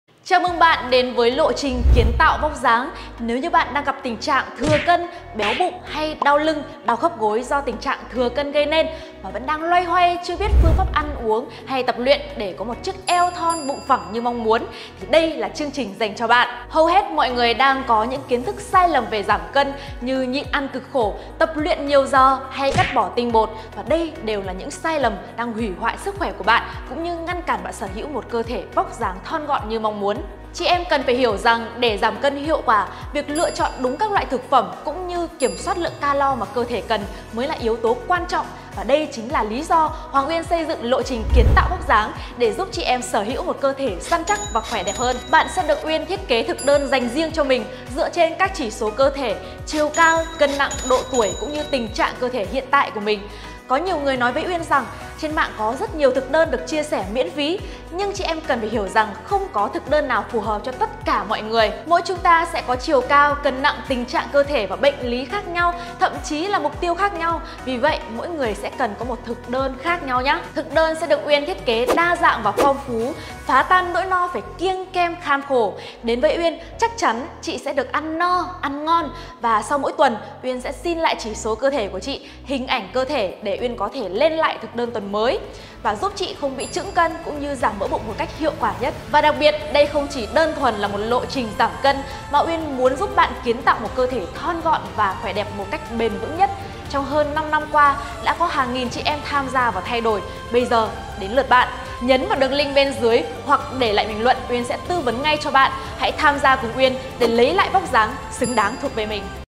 Lộ trình giúp bạn sở hữu eo thon bụng phẳng đầy quyến rũ | Hoàng Uyên Yoga
Hầu hết mọi người đang có những kiến thức sai lầm về g.iảm c.ân như nh.ịn ăn cực khổ, tập luyện hàng giờ, hay cắt bỏ tinh bột. Đây đều là những sai lầm đang hủy hoại sứ.c k.hỏe và ngăn cản bạn sở hữu một v.óc d.áng th.on gọn như mong muốn.
Chị em cần phải hiểu rằng để g.iảm c.ân hiệu quả việc lựa chọn đúng các loại thực phẩm và kiểm soát lượng calo mà cơ thể cần mới là yếu tố quan trọng. Đây chính là lý do Hoàng Uyên xây dựng lộ trình Kiến Tạo Vóc Dáng để giúp chị em sở hữu một cơ thể săn chắc và khỏe đẹp hơn.
Bạn sẽ được Uyên thiết kế thực đơn dành riêng cho mình dựa trên các chỉ số cơ thể. Thực đơn sẽ được Uyên thiết kế đa dạng và phong phú phá tan nỗi lo phải ăn kiêng kham khổ. Đến với Uyên bạn chỉ cần ăn ngon ăn no và khỏe đẹp hơn mỗi ngày.
Hãy tham gia cùng Uyên để lấy lại vóc dáng xứng đáng thuộc về mình.
Chat với Uyên để được tư vấn thêm về lộ trình!